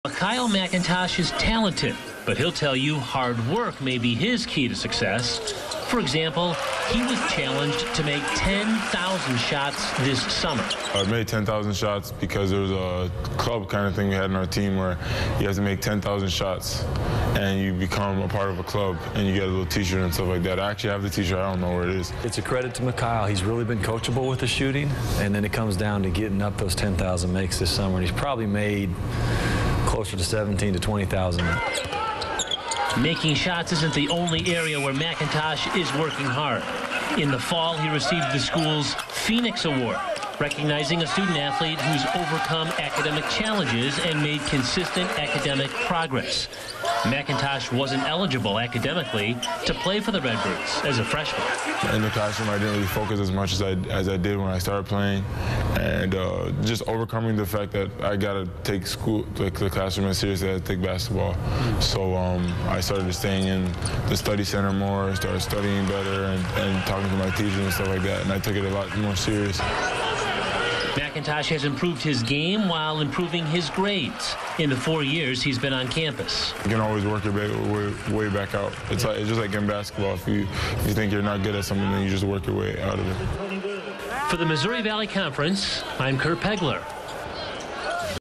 Profile: MiKyle McIntosh
Illinois State junior forward MiKyle McIntosh is second on the team in scoring, averaging 13.6 ppg this season.